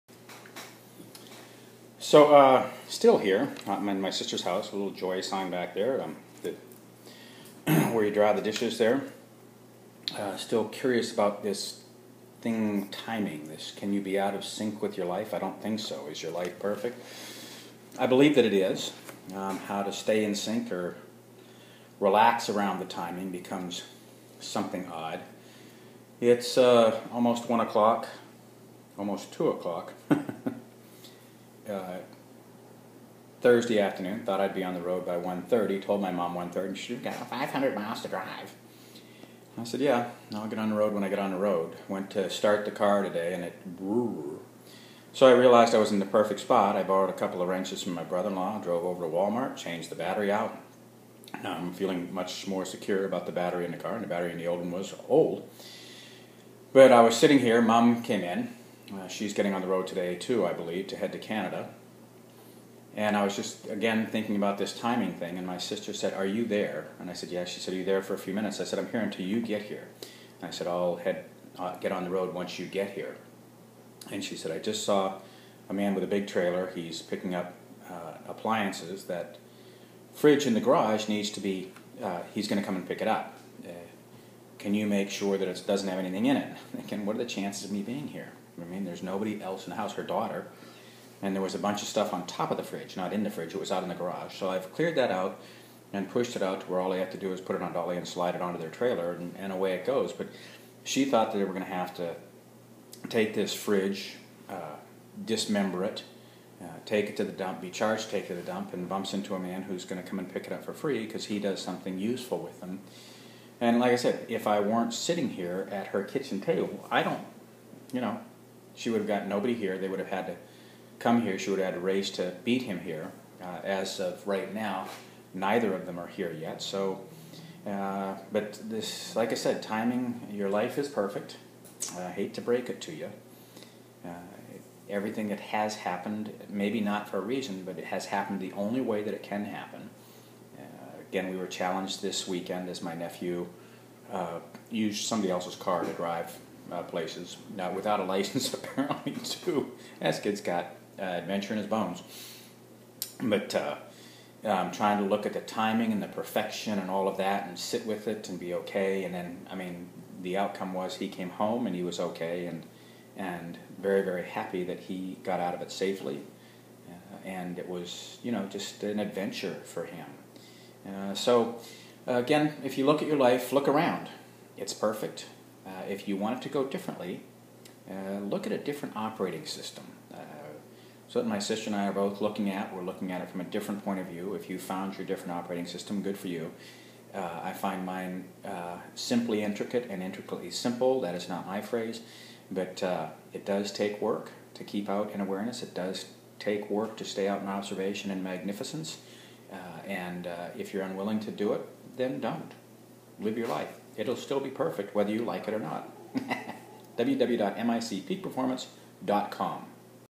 Perfect Life • Timing • Self-Actualization • Mick MacKenzie
Can you be 'out of sync' with your life?? Your life is perfect...